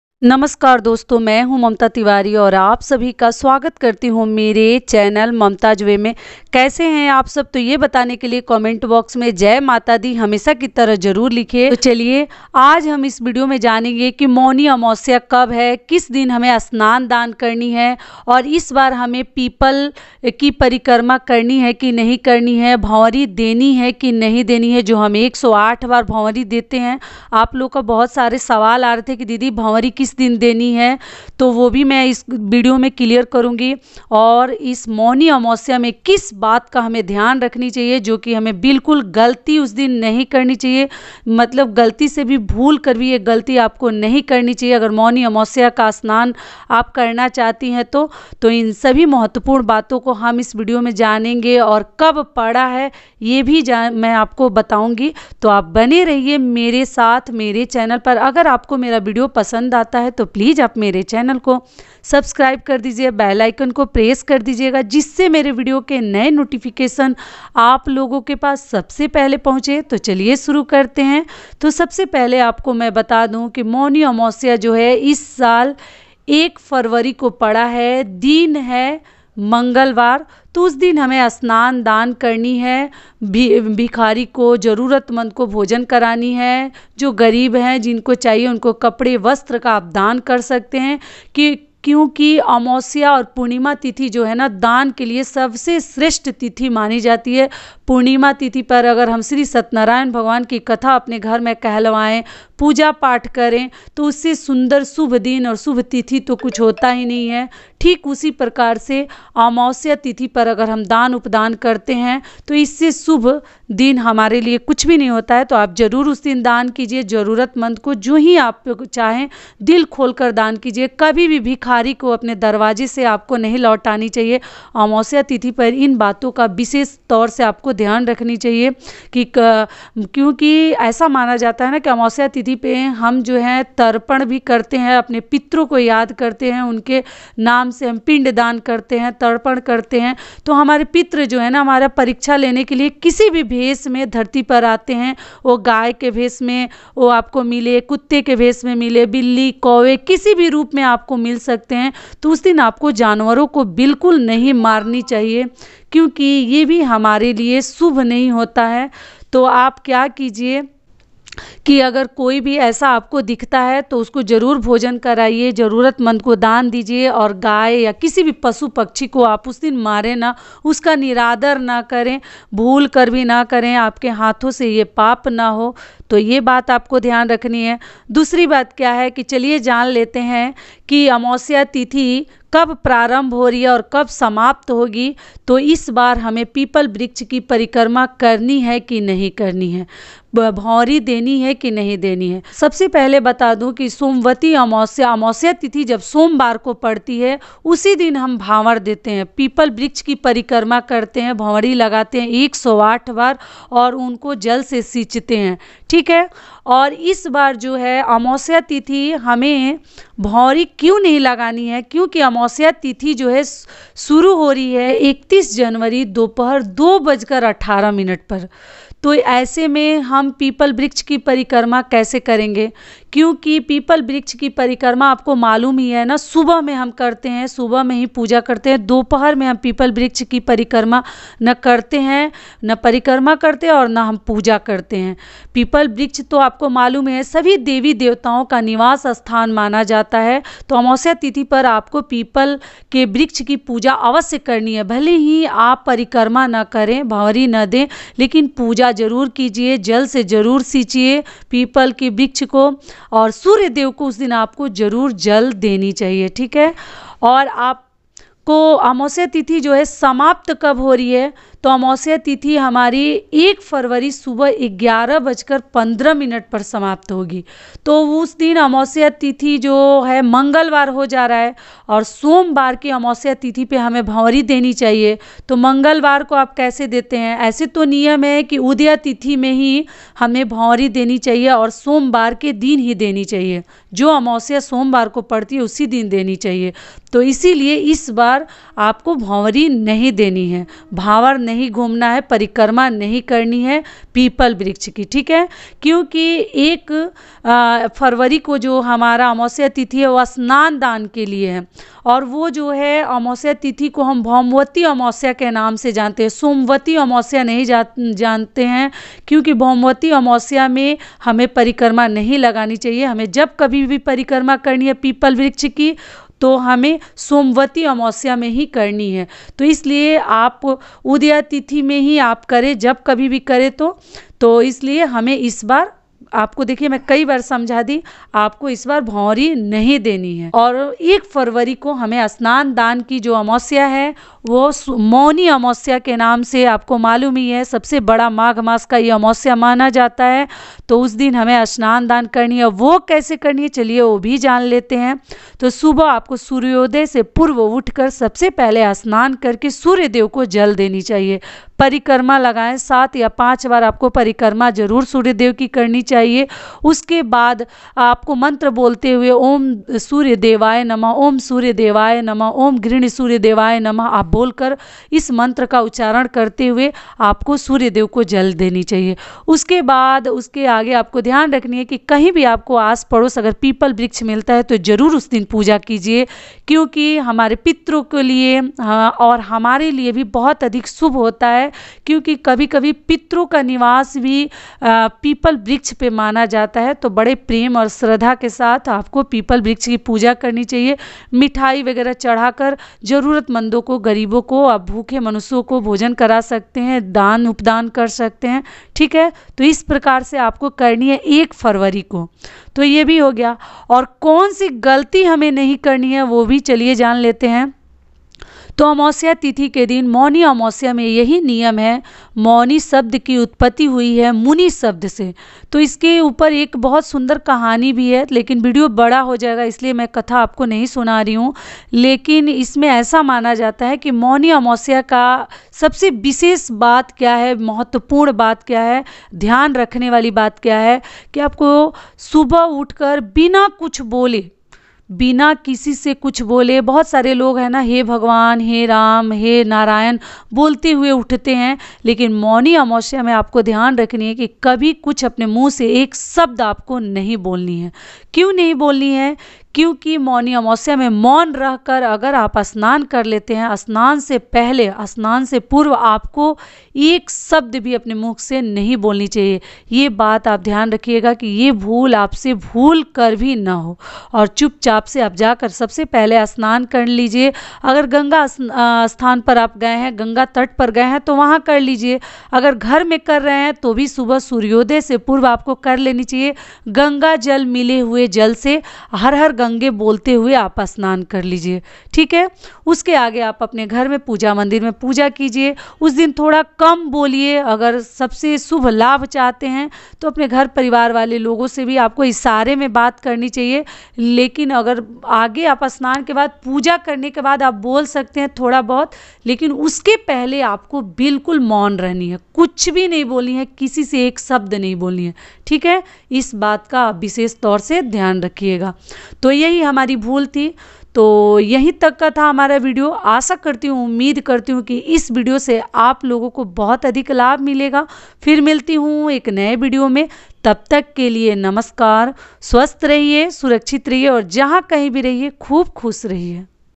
मौनी अमावस्या कब है। इस साल भावर दे या नहीं। माघी अमावस्या पूजा विधि। Mauni amavasya Kab hai 2022
Mauni amavasya
Maggi amavasya
somvati amavasya
amavasya kab hai
Mauni amavasya kab hai
amavasya Ko kya Karen kya Nahin